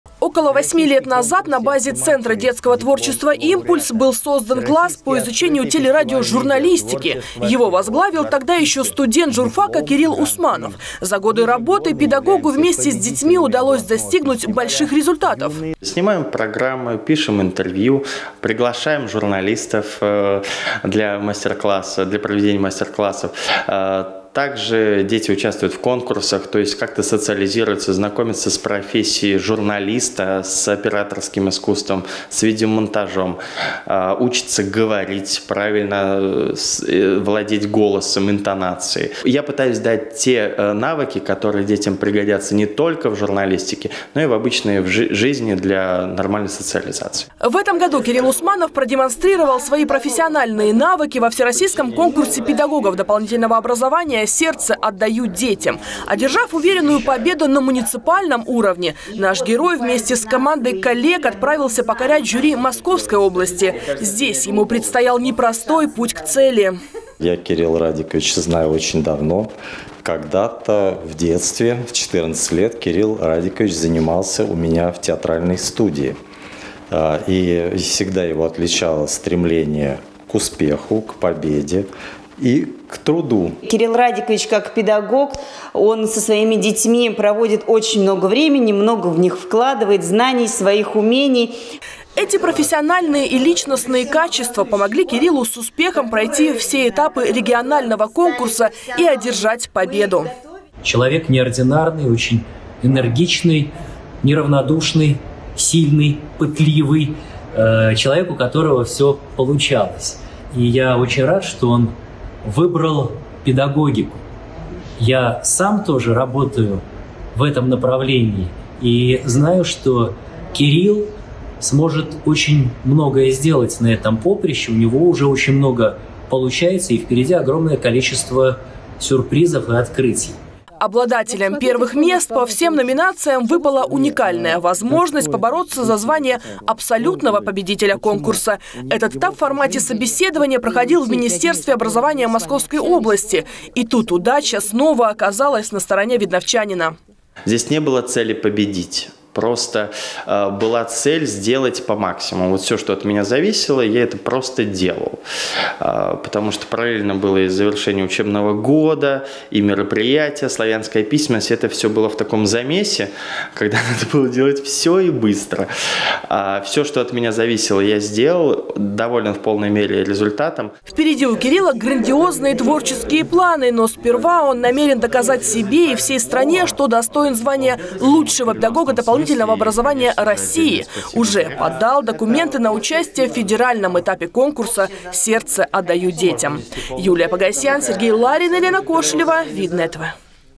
АБСОЛЮТНЫЙ ПОБЕДИТЕЛЬ
В Московской области завершился региональный этап всероссийского конкурса «Сердце отдаю детям», абсолютным победителем которого стал видновчанин, педагог центра детского творчества «Импульс» Кирилл Усманов. С лучшим учителем дополнительного образования встретилась  наш корреспондент Юлия Погосьян.